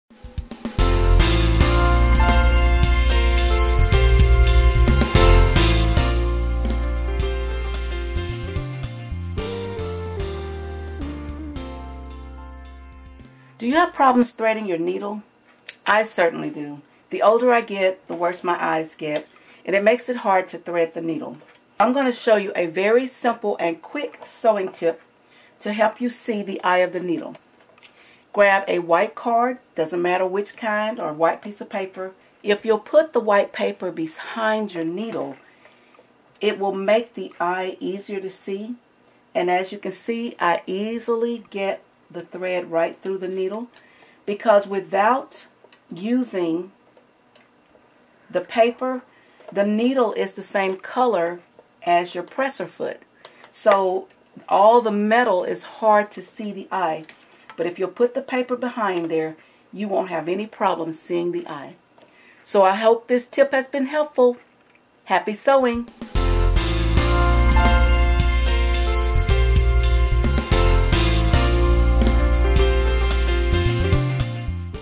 How To Thread Sewing Machine Needle Quick & Easy
Can't sew your sewing machine needle?  Neither can I.  Here's a tip that works wonders with me.  Using a simple piece of white paper can make all the difference in the world.  Watch this video and see just how quickly you can thread your needle...Happy Sewing!